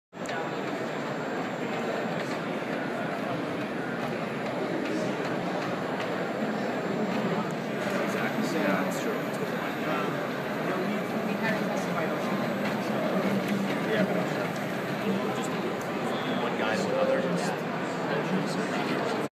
CES 2012: 3D Printer
From CES 2012, a 3D Printer from MakerBot Industries. Creates actual physical products.